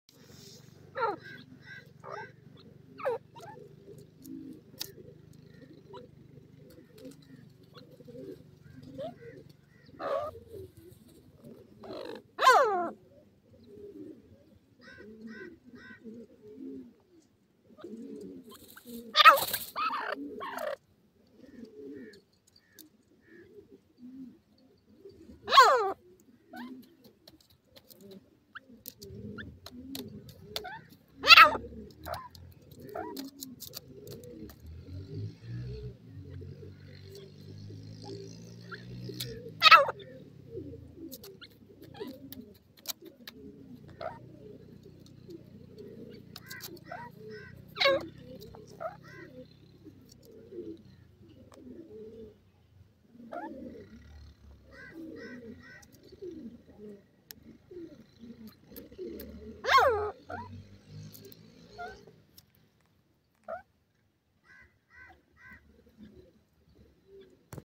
baby pheasants get tired after playing,bird video teetar
Experience the delicate snapshots of child fowls and Teetar chicks as they play and rest together in the magnificence of nature!

In this endearing video, witness the cute sight of child birds and Teetar chicks as they skip around and play with vast energy before at long last capitulating to weariness
 From their lively tweets to their awkward bounces, each second catches the honesty and appeal of these youthful birds.

As the day unfurls, watch as these small animals investigate their environmental factors, take part in perky shenanigans, and at last track down comfort in the soothing hug of rest. Their process helps us to remember the significance of rest and revival amidst life's happy minutes.

Go along with us as we praise the magnificence of nature's rhythms and the valuable obligations of kinship produced between these padded mates
 Through their perky connections and serene sleep, may we track down motivation to esteem the basic delights of life.